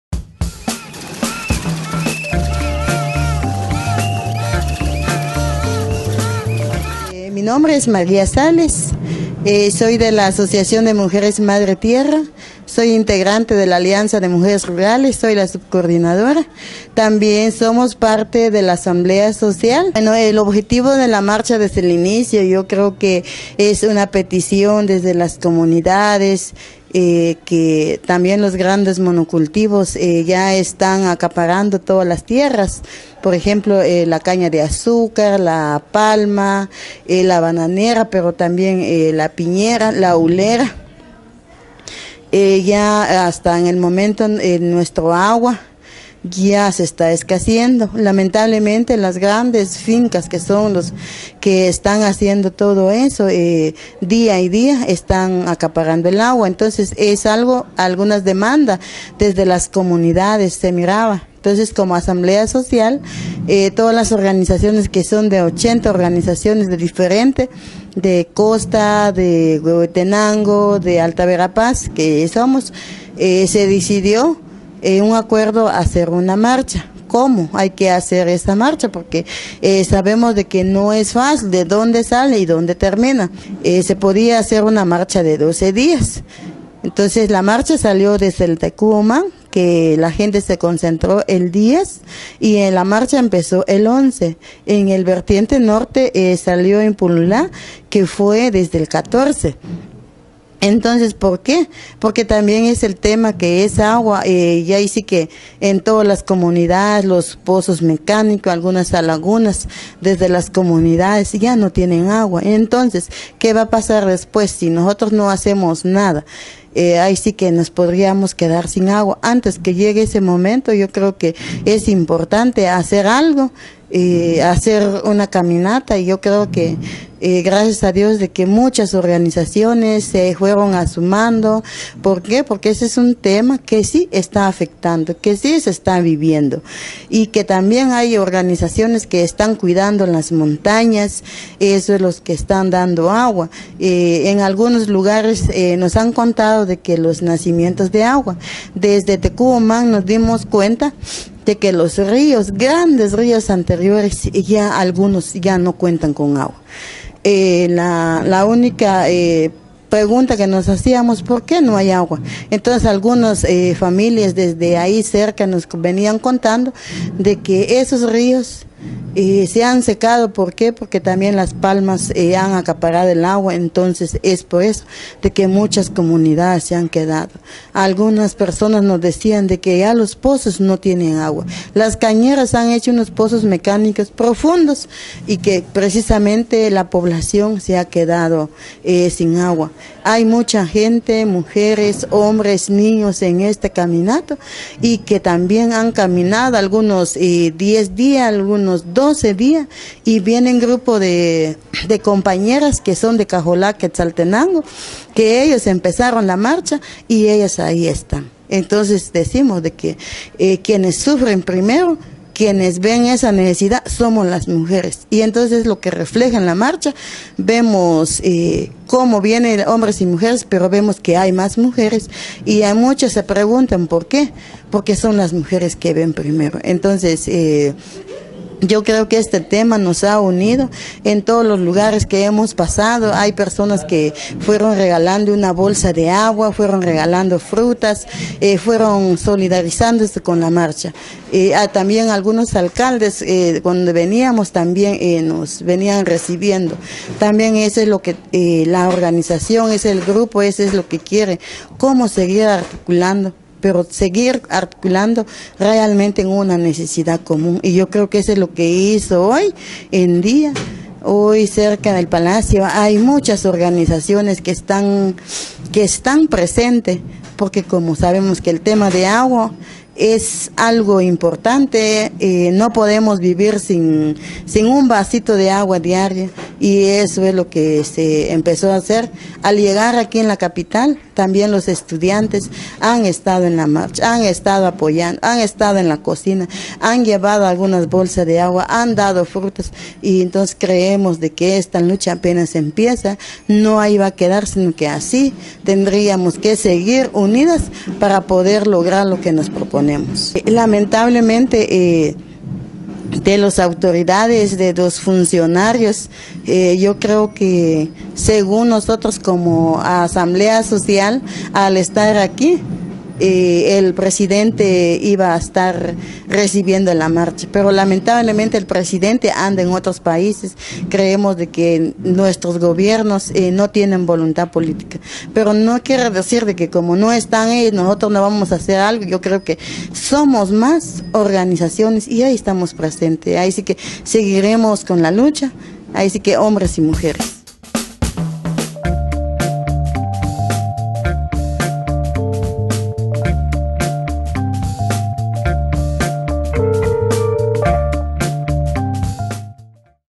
María Sales
María Sales no platica unos minutos sobre la MarchaPorElAguaGt